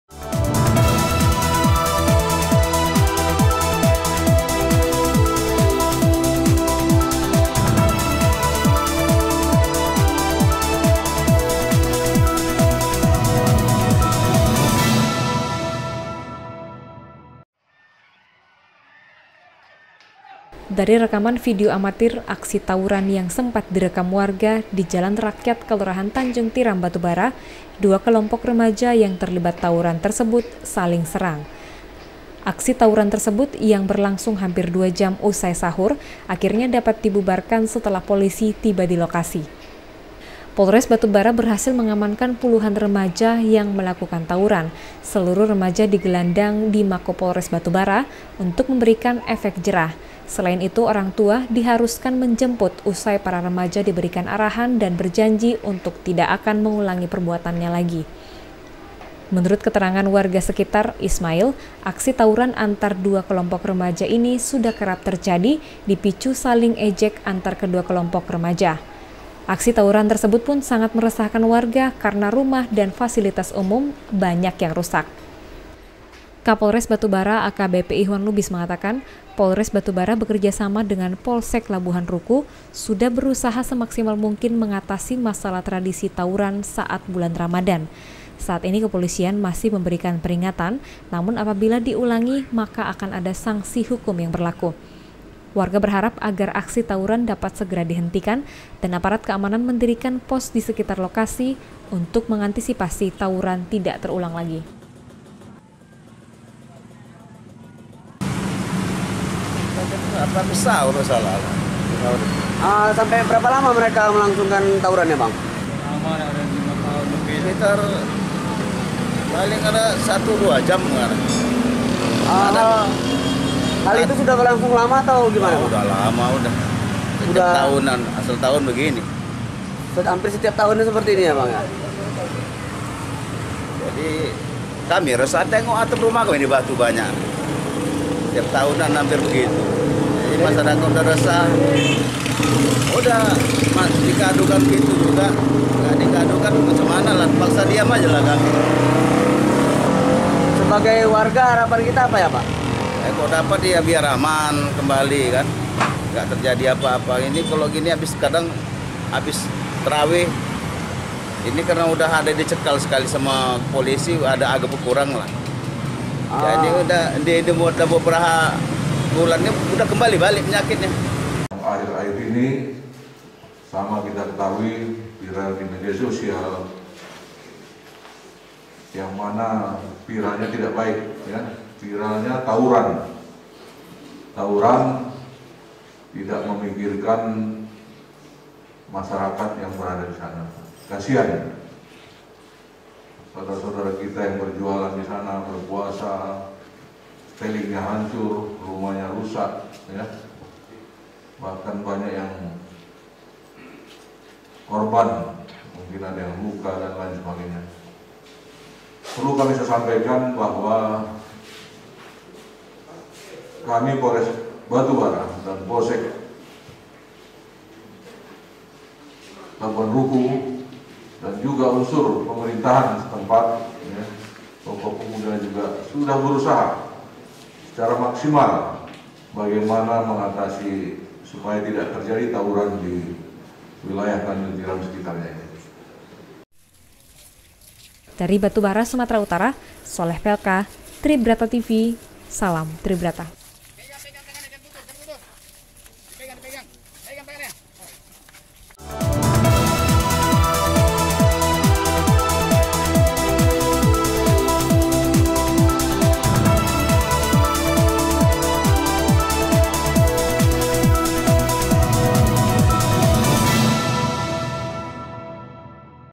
PULUHAN REMAJA DI BATUBARA DIAMANKAN POLISI KARENA TAWURAN
Dari rekaman video  amatir aksi tawuran yang sempat direkam warga di kelurahan Tanjung Tiram Batubara dua kelompok remaja yang terlibat tawuran tersebut saling serang. Aksi tawuran tersebut yang berlangsung hampir dua jam usai sahur, akhirnya dapat  dibubarkan setelah polisi tiba di lokasi.

Polres Batu Bara berhasil mengamankan puluhan remaja yang melakukan tawuran. Seluruh remaja digelandang di Makopolres Batu Bara untuk memberikan efek jera. Selain itu orang tua anak di haruskan menjemput usai para remaja diberi arahan dan berjanji untuk tidak akan mengulangi perbuatannya lagi.

Menurut keterangan warga sekitar Ismail, aksi tawuran antar dua kelompok remaja ini sudah kerap terjadi dipicu saling ejek antar kedua kelompok remaja tersebut. Aksi tawuran tersebut pun sangat meresahkan warga karena rumah warga dan fasilitas umum yang ada banyak yang rusak.
Kapolres Batu Bara AKBP Ikhwan Lubis mengatakan Polres Batu Bara bersama dengan Polsek Labuhan Ruku sudah berusaha semaksimal mungkin mengatasi masalah tradisi tawuran saat bulan Ramadhan saat ini kepolisian masih memberikan peringatan namun apabila diulangi lagi maka akan ada sanksi hukum yang berlaku.

Warga berharap agar aksi tawuran dapat segera dihentikan dan aparat keamanan mendirikan pos di sekitar lokasi untuk mengantisipasi tawuran tidak terulang kembali.

Dari Batu Bara Sumatera Utara Sholeh Pelka Tribrata Tv salam Tribrata.